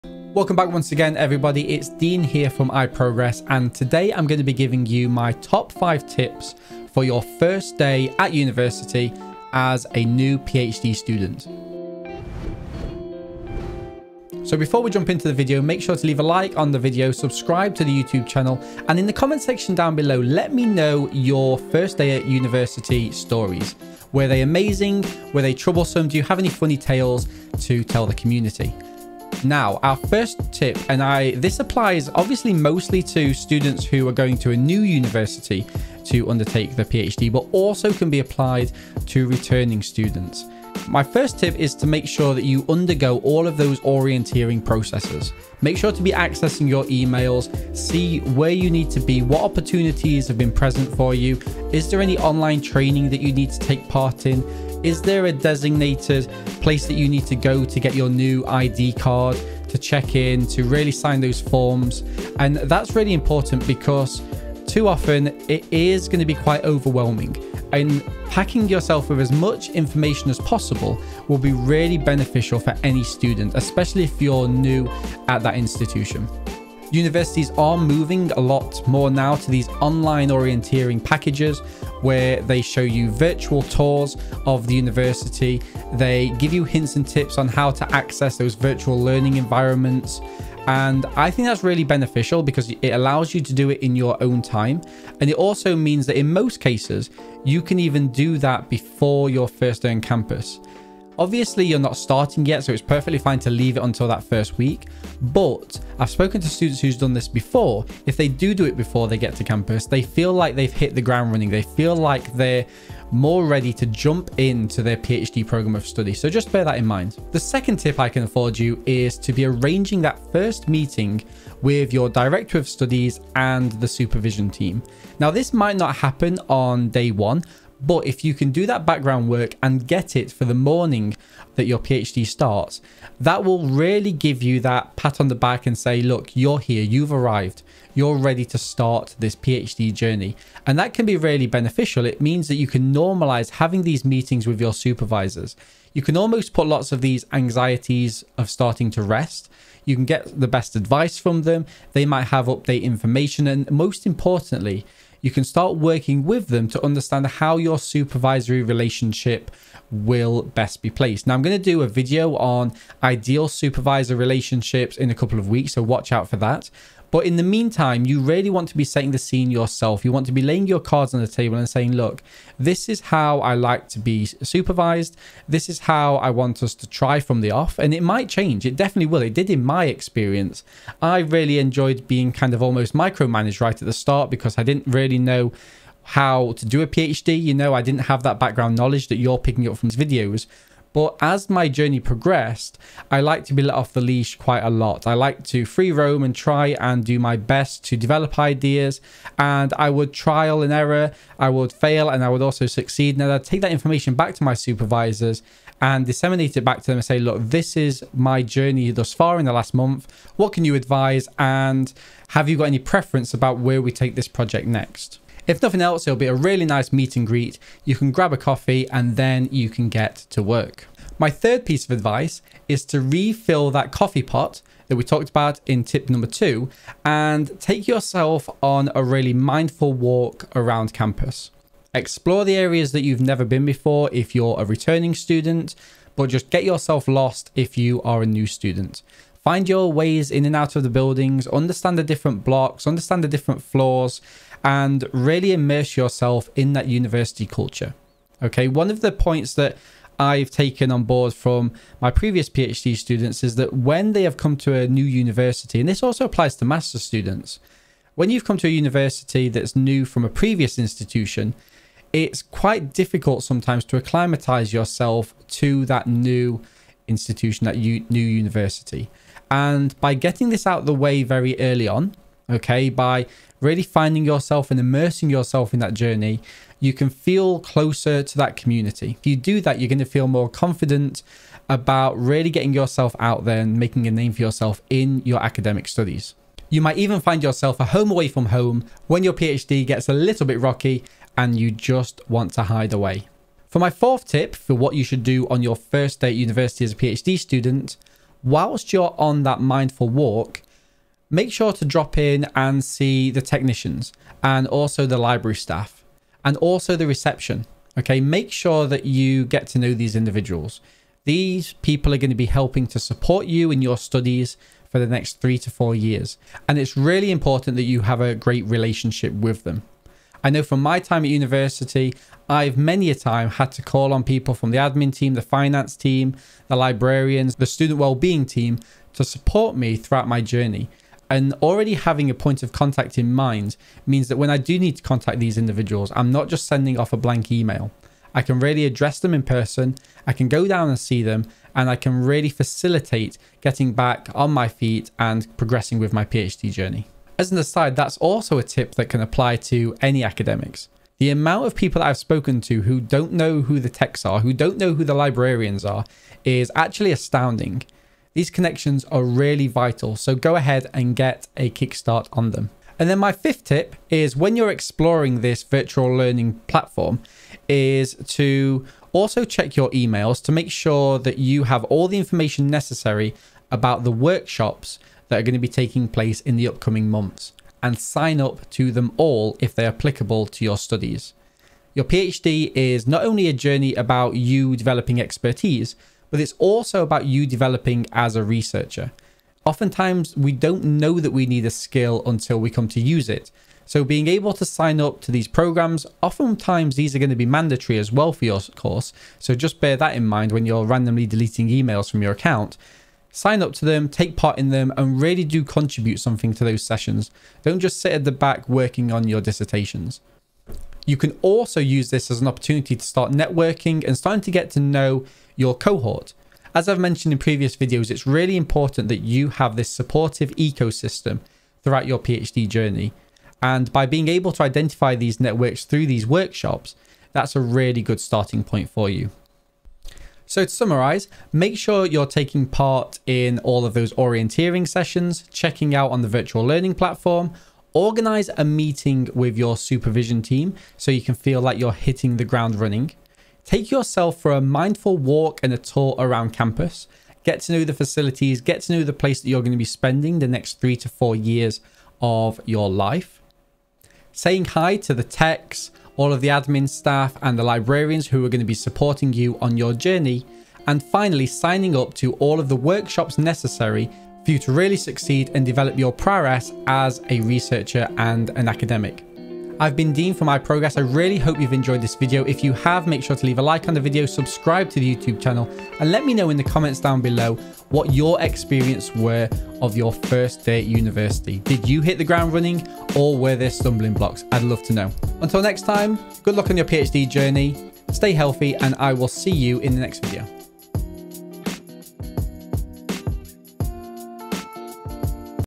Start Your PhD Well... ON DAY ONE!
Welcome back!  Remember to JOIN US ON DISCORD

In this video, Dr. Dean Fido suggests 5 things to do on the first day of your PhD!

CHAPTERS:
00:00 - Introduction
00:40 - Undergo Orientation
01:56 - Arrange 1st Supervision Meeting
03:40 - Go on a Campus Walk
04:55 - Meet Support Staff
05:55 - Sign up to Workshops